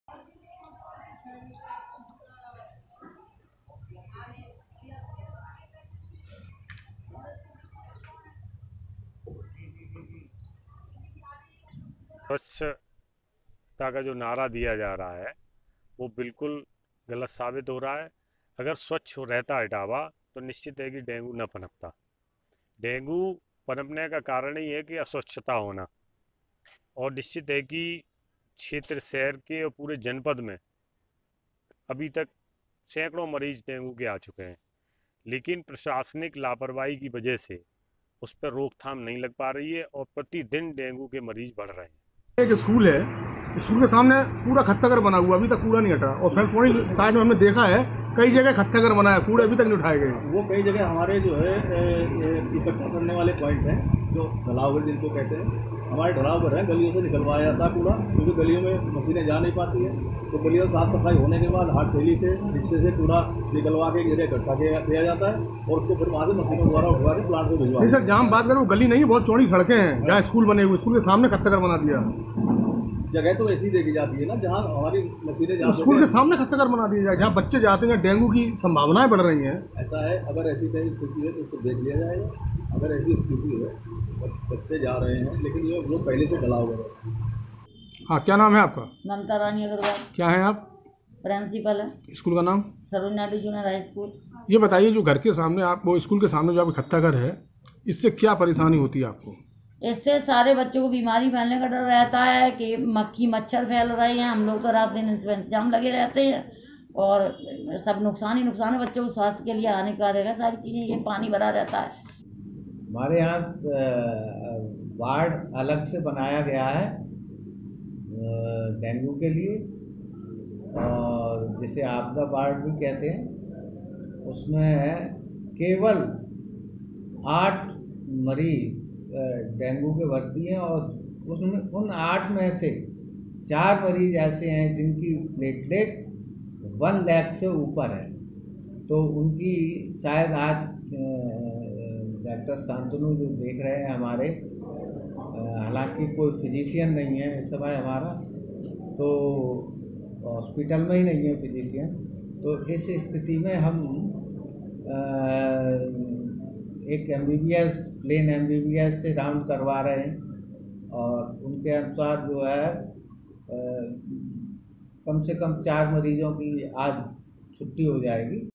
इटावा : ISO प्रमाणित शहर में लगा गंदगी का अंबार, डेंगू ने दी दस्तक
इटावा: मुख्यमंत्री योगी आदित्यनाथ ने जिस इटावा शहर को स्वच्छता का आईएसओ प्रमाणपत्र दिया था. वही शहर गंदगी से पटा पड़ा है. अब शहर में डेंगू जैसी खतरनाक बीमारी ने दस्तक दी है. इससे तो यही लगता है कि जिला प्रशासन ने डेंगू को रोकने के लिए कोई प्रयास अब तक नहीं किए.